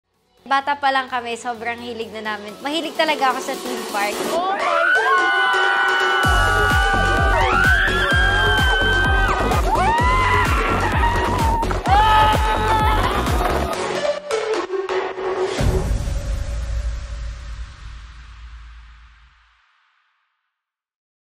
Mika and Nash (Channel Trailer)
Matagal na namin pinagpaplanuhan itong step ng pagtatayo ng sarili naming couple channel and we are finally launching it!

We are so excited to share this with you because this is not your typical vlog, this is a passion project fueled by Nash’s eye for directing and my passion for vlogging combined. From pre-prod to camera to editing to post-prod, kami lang po ito! 🙈 We put our hearts, time and effort to create content that you guys deserve! 🤗

2020 was a bad year for all of us and that pushed us to create this series with the purpose of turning last year’s negativity into this year’s positivity.

And through our “PROJECT HAPPINESS”, we hope you find inspiration as we go on this journey of healing and self-growth. 🤍🥺